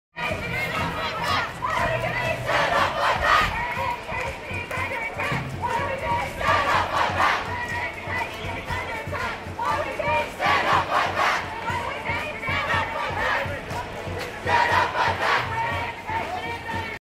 Education chant on National Student Strike and staff march in Melbourne
14th of May was the date of a national student strike called by the National Union of Students, and held in conjunction with the National Tertiary Education Union who called for a staff rally. The rally was a response to the Gillard Government's 2.3 billion in cuts to Higher Education, and was called for Federal Budget day. Around 2000 students and staff marched from State Library to Federation Square. Here we hear them chant 'When education is under attack, what do we do? Stand up! Fight Back!', on Bourke and Swanston St.